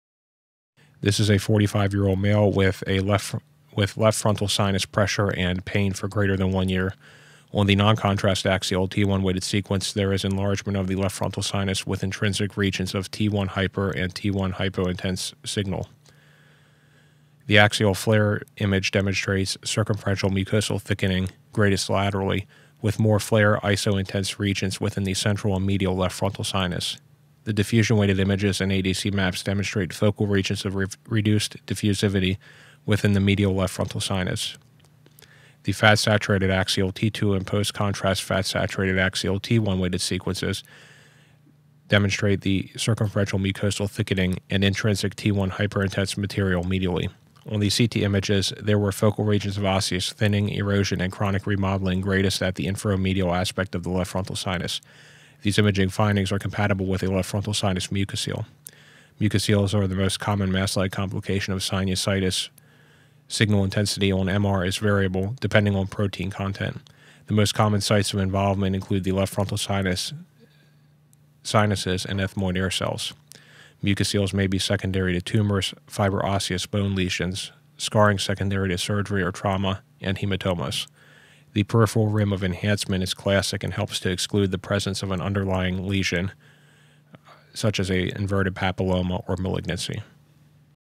Mucocele
This is a case of a left frontal sinus mucocele in a 45-year-old male presenting with signs and symptoms of sinusitis for &gt; 1 year. Image 1, a noncontrast axial T1-weighted sequence, demonstrates enlargement of the left frontal sinus with regions of intrinsic T1-hyper and hypointense signal. Image 2, an axial FLAIR sequence, demonstrates circumferential mucosal thickening, greatest laterally, with more FLAIR isointense signal within the central/medial left frontal sinus. The DWI and ADC maps, on images 3 and 4, demonstrate focal regions of reduced diffusivity within the medial aspect of the left frontal sinus. The fat-saturated axial T2 and post contrast fat-saturated axial T1-weighted sequences, images 5 and 6, redemonstrates the circumferential mucosal thickening and intrinsic T1-hyperintense material medially.These findings are classic for a left frontal sinus mucocele, which is the most common masslike complication of sinusitis. The variable protein content within the mucocele leads to variable signal intensity on the T1- and T2-weighted sequences. The most common sites of involvement are the frontal sinuses, ethmoid air cells, maxillary antra and sphenoid sinuses, in decreasing order of frequency.